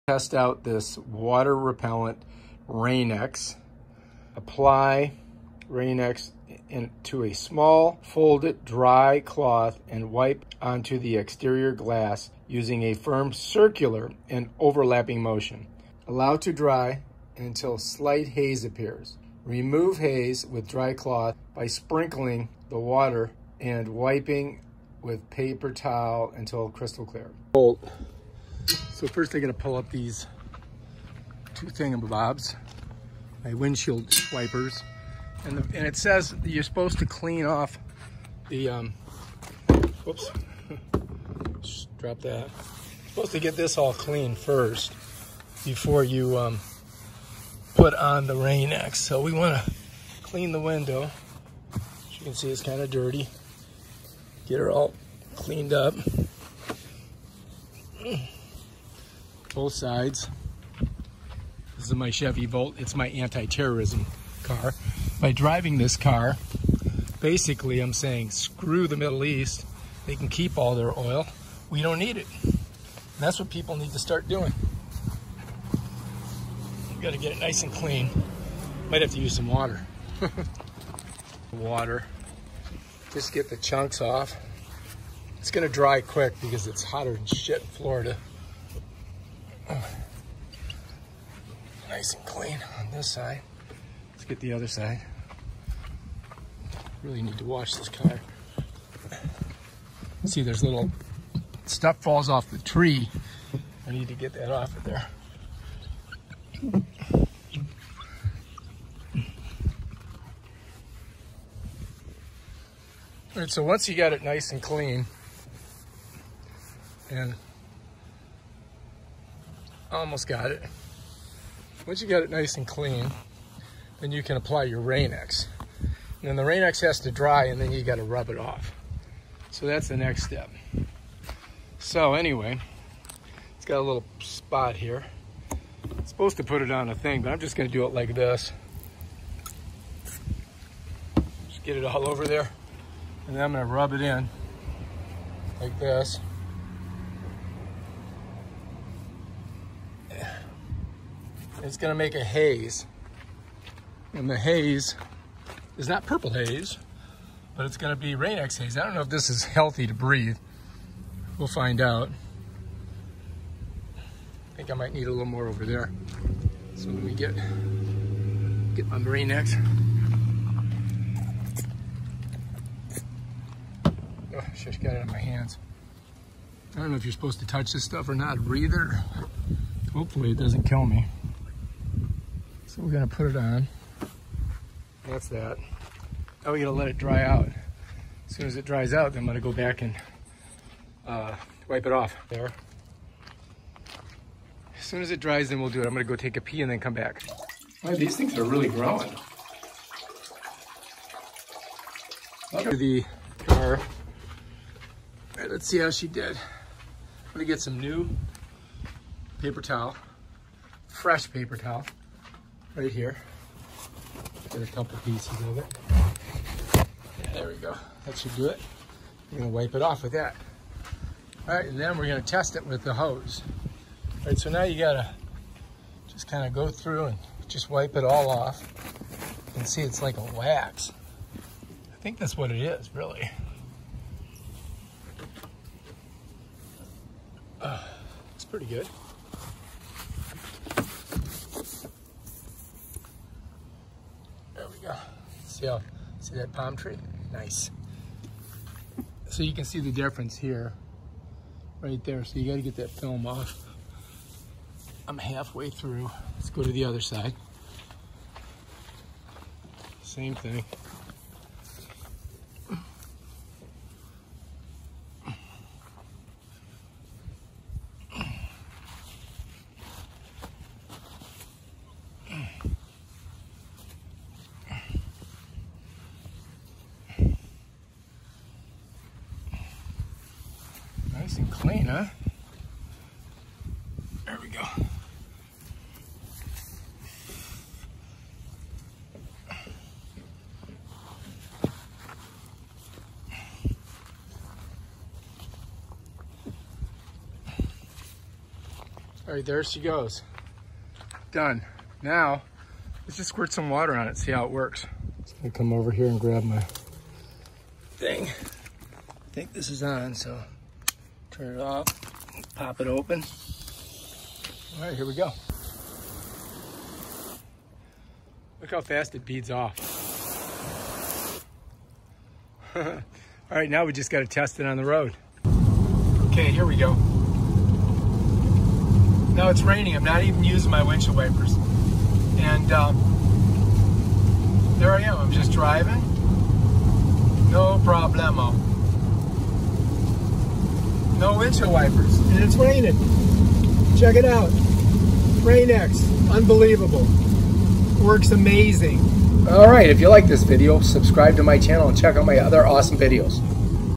Does Rain-x work?!
Rain-x works! I used it and I did not need my windshield wipers!